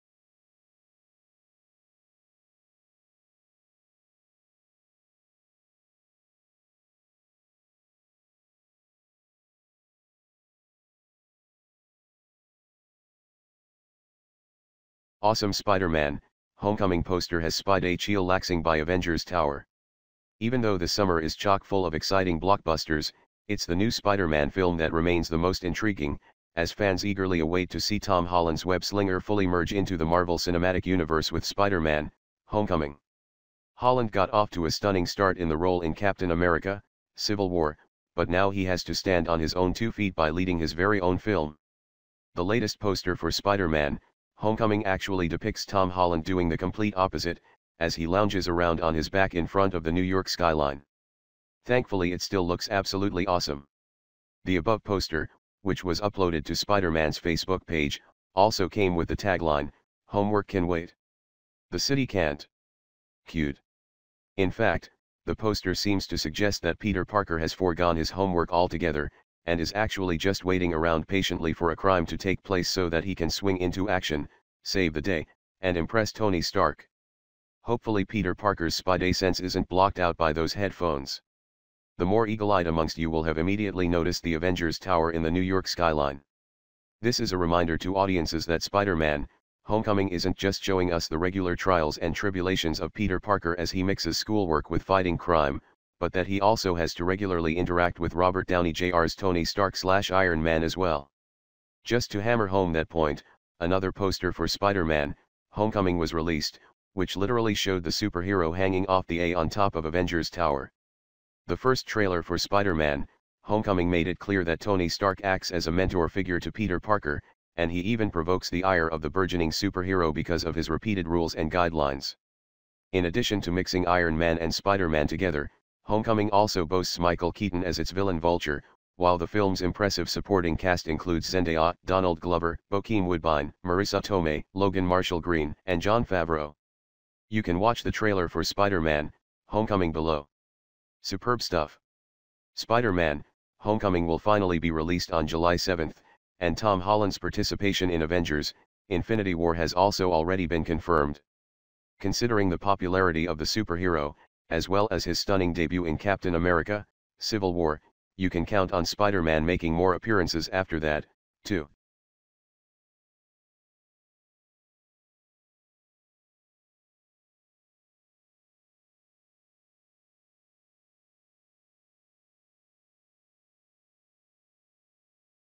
Awesome Spider-Man: Homecoming Poster Has Spidey Chillaxing By Avengers Tower
Even though this summer is chock full of exciting blockbusters, it's the new Spider-Man film that remains the most intriguing, as fans eagerly await to see Tom Holland's web-slinger fully merge into the Marvel Cinematic Universe with Spider-Man: Homecoming. Holland got off to a stunning start in the role in Captain America: Civil War, but now he has to stand on his own two feet by leading his very own film. The latest poster for Spider-Man: Homecoming actually depicts Tom Holland doing the complete opposite, as he lounges around on his back in front of the New York skyline. Thankfully it still looks absolutely awesome.